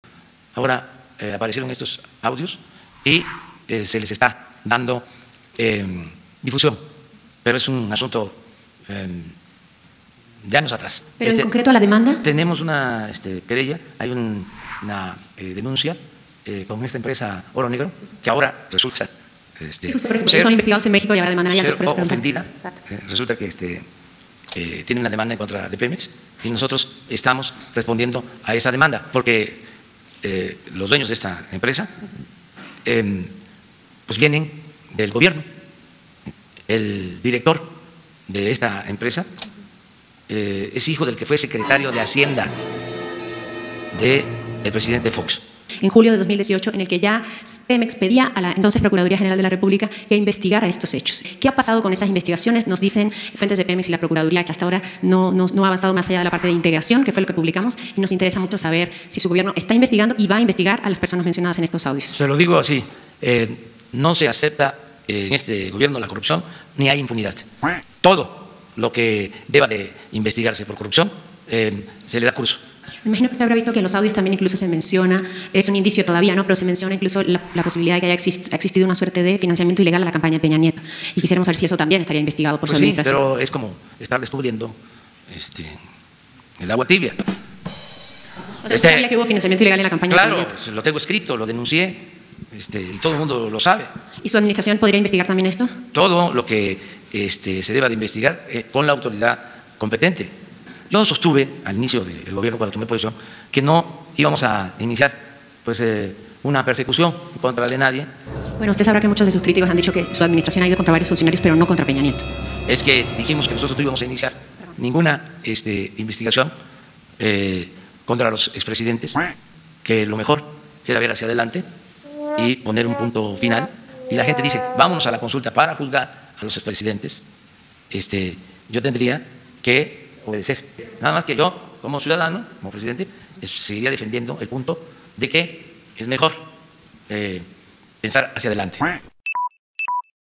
Oro Negro vs PEMEX; Financiamiento ilegal a campaña de EPN... pero mejor punto final
Hoy habló sobre Oro Negro y la demanda en contra de PEMEX, también recordó que fue él quién denunció públicamente el financiamiento ilegal de la campaña de Enrique Peña Nieto... pero sostiene que no habrá persecución a ningún expresidente a menos que el pueblo se lo pida